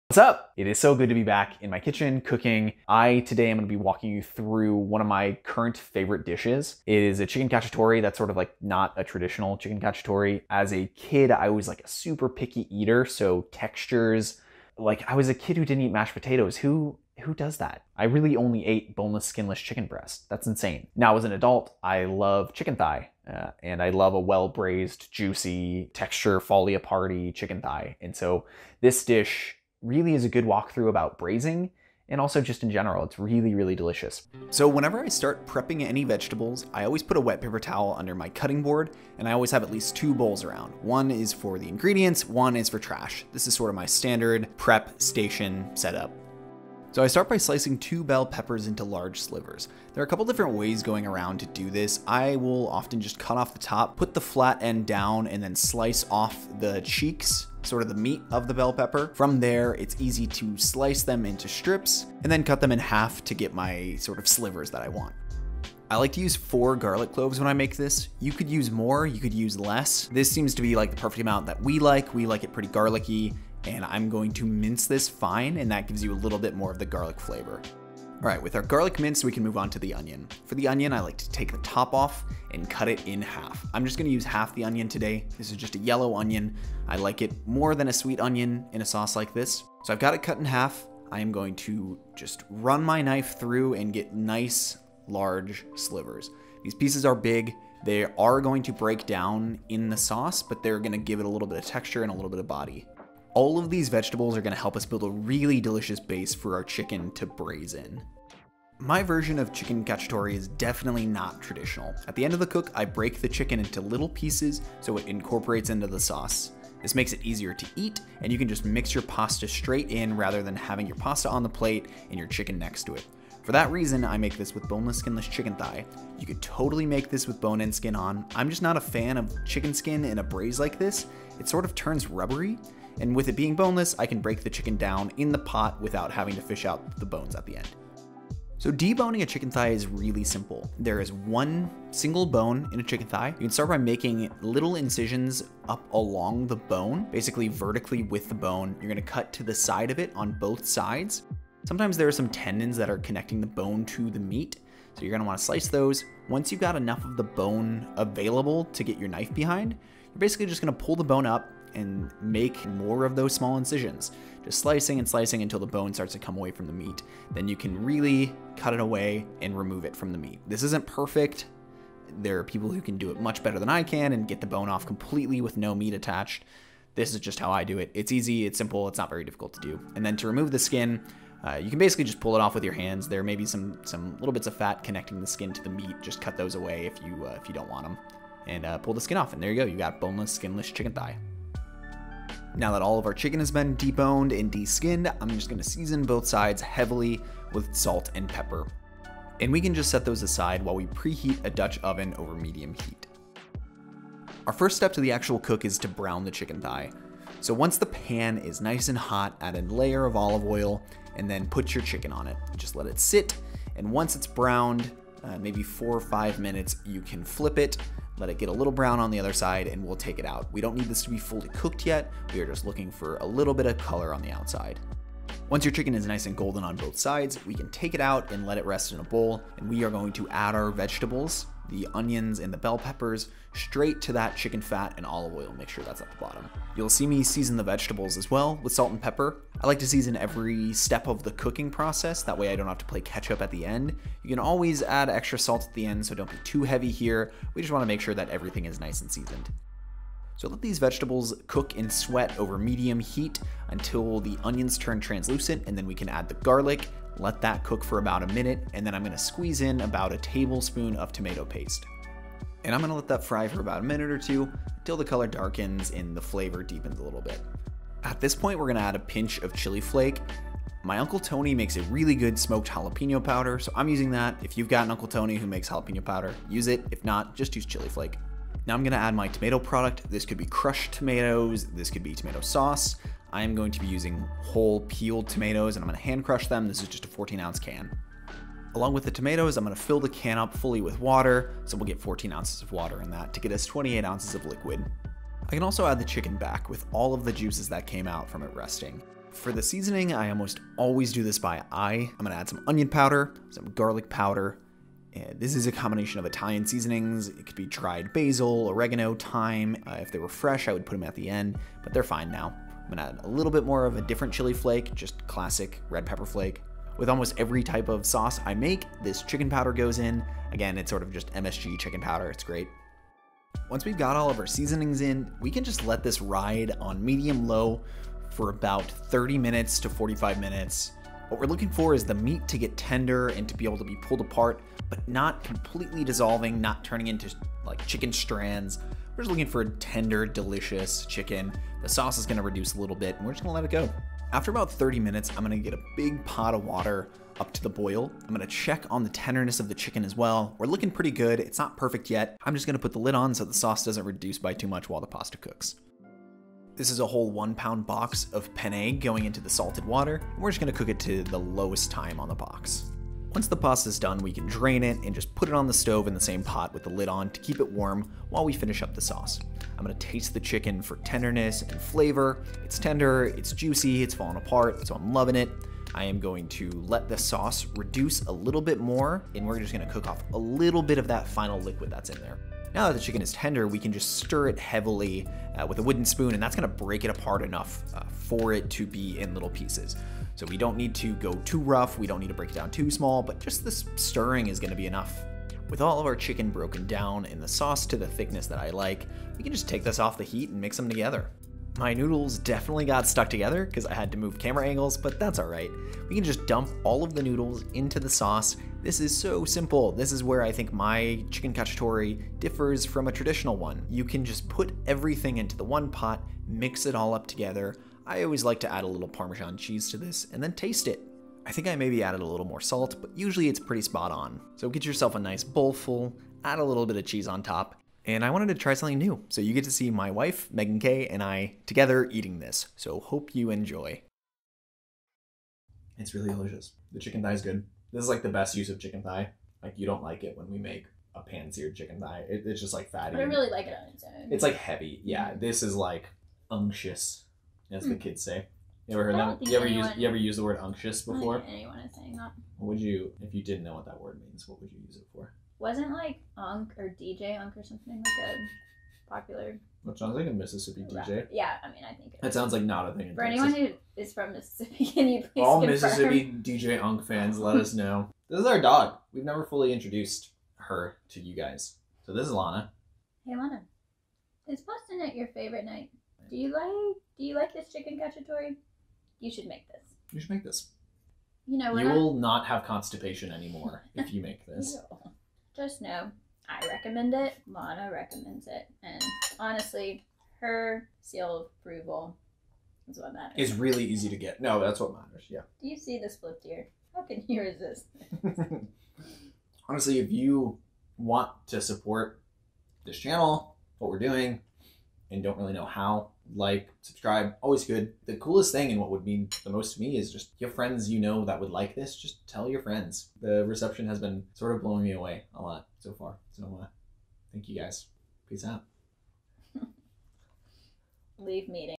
Tired of Spaghetti? Try my Chicken Cacciatore recipe
When you want a big bowl of warm, comforting goodness, this is the dish you want to make. It is coziness in a bowl that is perfect for a Sunday dinner. Some traditional Cacciatore recipes include mushrooms, olives and any number of other vegetables. I keep things simple with ingredients I regularly have on hand, but you can probably get away with adding any vegetables you like!

All the equipment I like!

Link to full written recipe!

---Ingredients---

1lb boneless skinless chicken thigh
14 oz can crushed or whole tomato
14 oz water
2 bell peppers
½ yellow onion
4 garlic cloves
1 tbsp tomato paste
1 tsp garlic powder
1 tsp onion powder
1 tsp dried herbs (basil, oregano, thyme, rosemary)
½ tsp chicken powder or bouillon
Chili flake to taste
1 lb dried pasta

When in Rome, mess with Italian recipes.